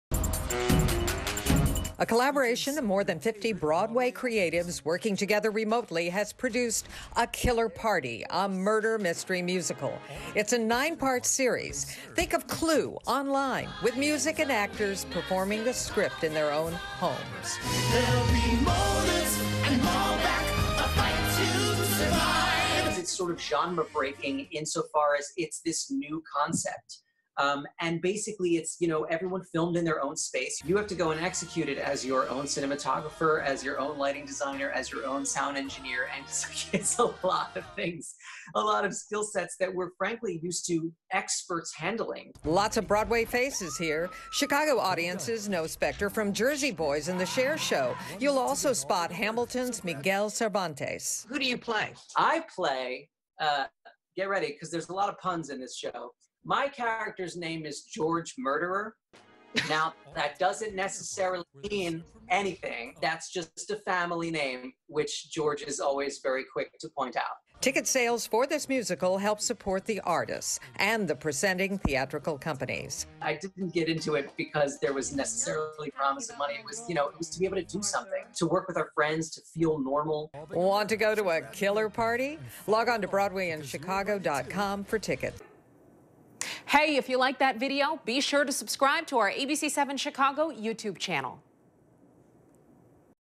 'A Killer Party- A Murder Mystery Musical,' 9-part series filled with Broadway faces premiers Wednes
'It's sort of genre breaking in so far it's this new concept.' FULL STORY